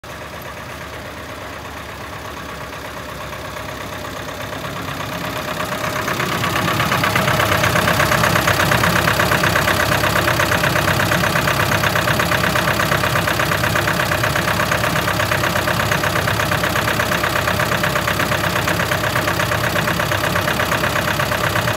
2018 Generac MMG25IF4 Self Contained Generator For Virtual Online Auction March 11, 2023
2018 Generac MMG25IF4 Self Contained Generator S/A 22KW, 22KVA Engine: 4cyl Isuzu Diesel S/N: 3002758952 Hours: 8029 (BAD TURBO)(OIL LEAK)

March 11, 2023 Public Virtual Online Auction. Auction Starts at 8:00 A.M No Deposit Required to Register & Bid Online, Online Bidding is open now. NO RESERVES! Preview is Monday through Friday from 7:00 AM to 4:00 PM Location: 1616 S. 67th Ave Phoenix, AZ 85043.